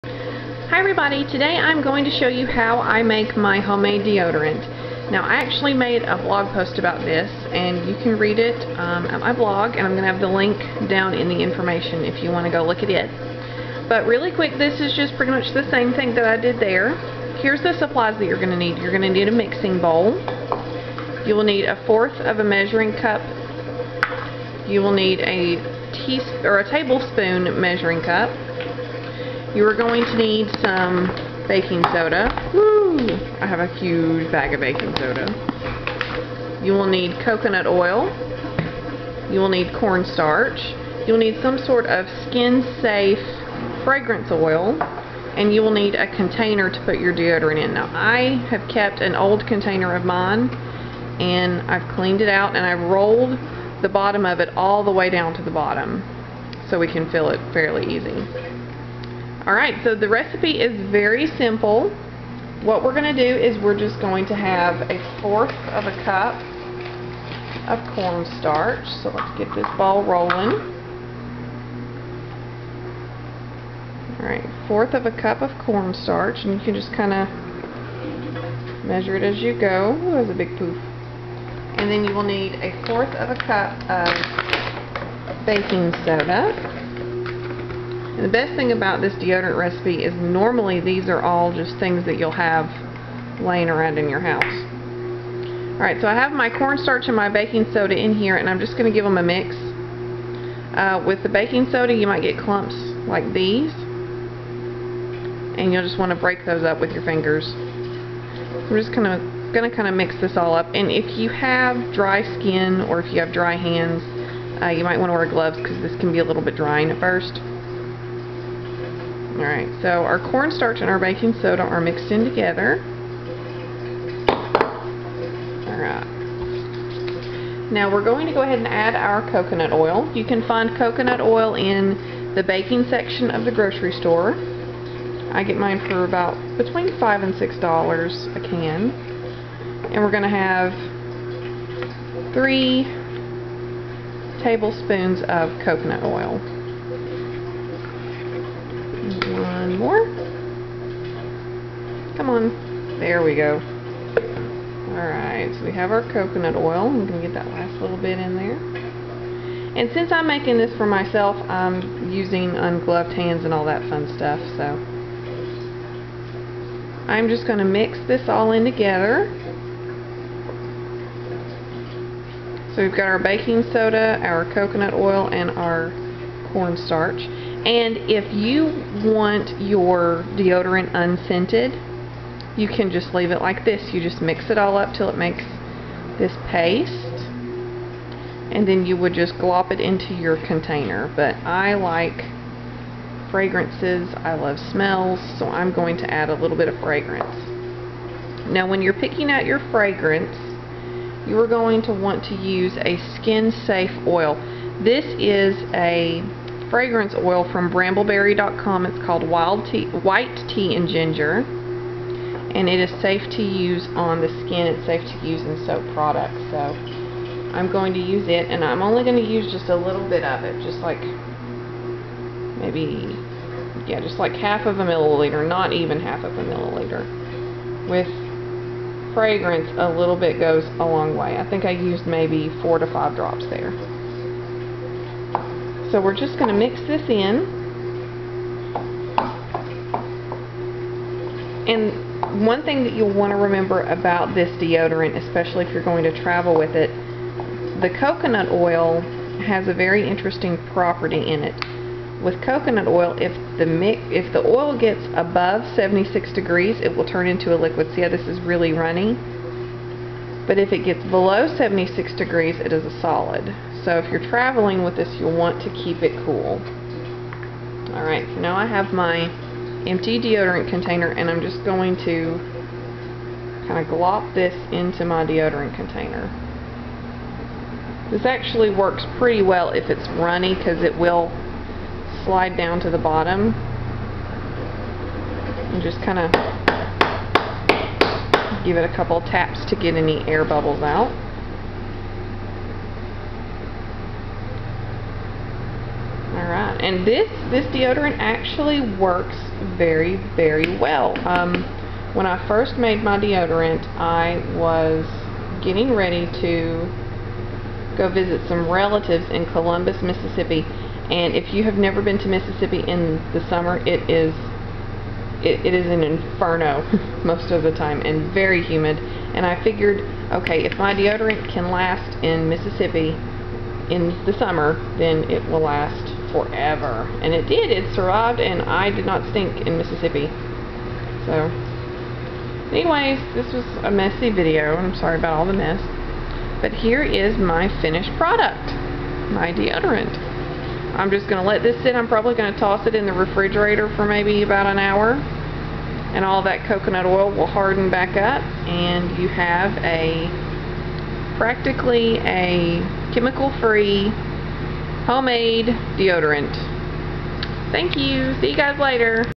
How to Make your own Deodorant
Make your own deodorant from stuff in your kitchen!! Chemical and aluminum free!